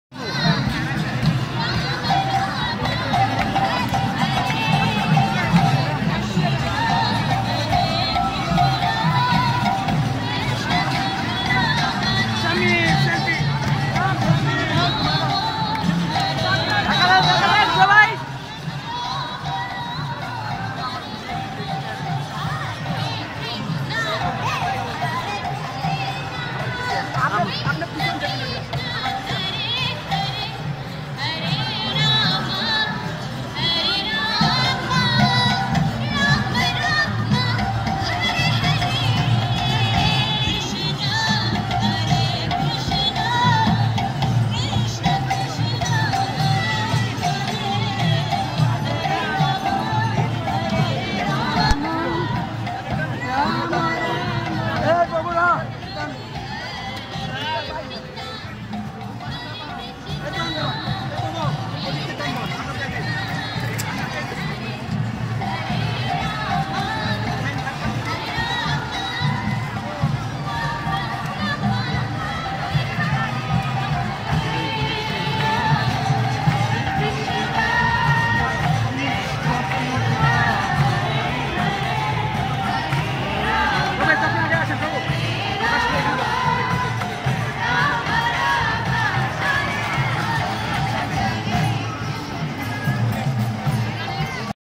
Ratha Yatra18
লক্ষ মানুষের মিলন মেলায় বাংলাদেশের রথ যাত্রা উদযাপন ২০১৮। আয়োজনে ইসকন।